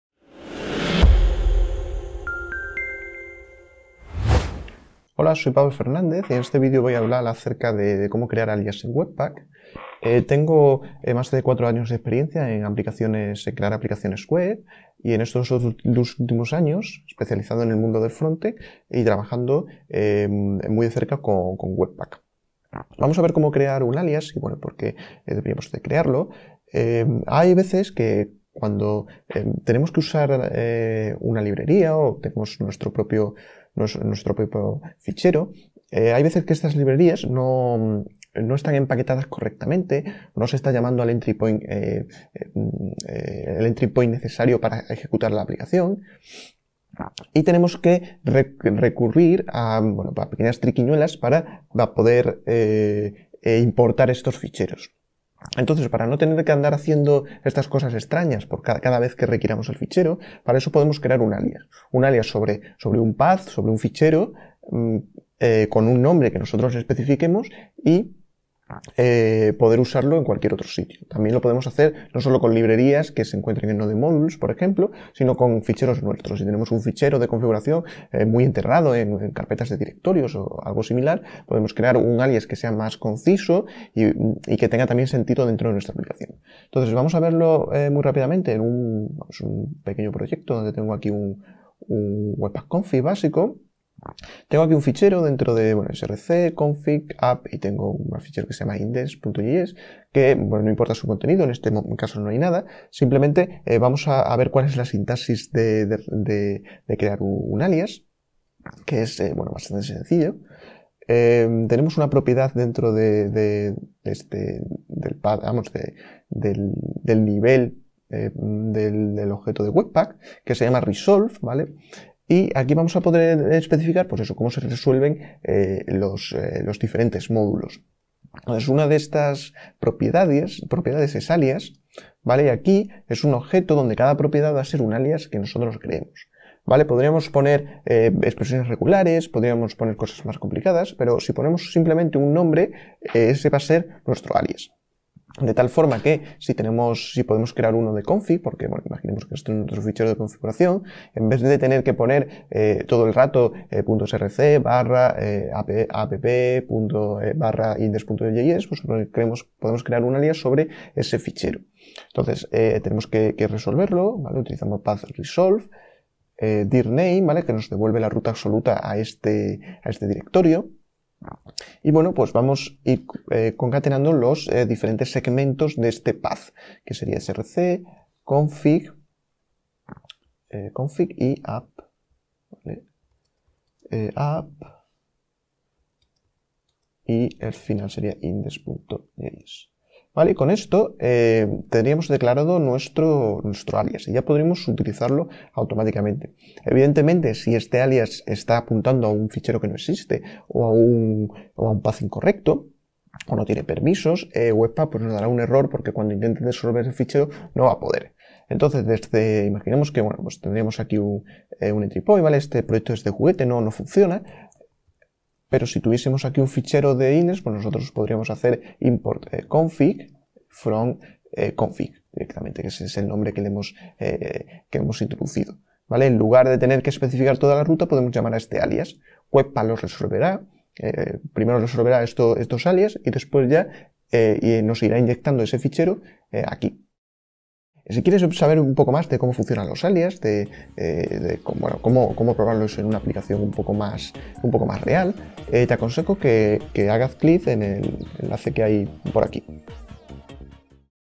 WEBPACK: CREAR UN ALIAS
INDICE:

01# Qué es un Alias en Webpack - 0:22
02# Ejemplo práctico de un Alias - 1:26